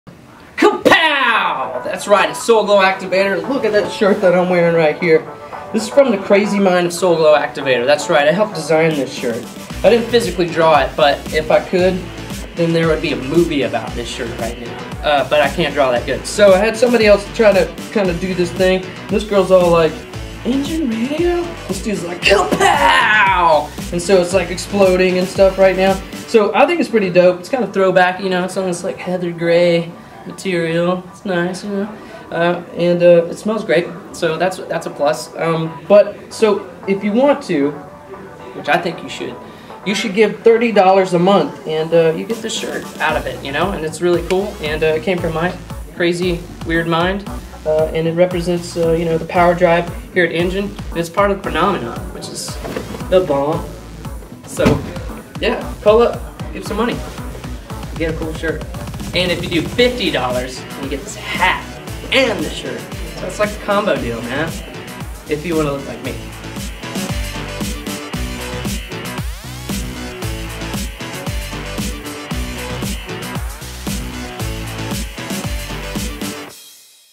From the Mind of Soul Glow Activatur: NGENPowerDrive Shirt
Soul Glow shares the creative process behind his design for this limited edition NGEN Power Drive shirt! Here's how you can Power A World of Good and add this fly tee to your wardrobe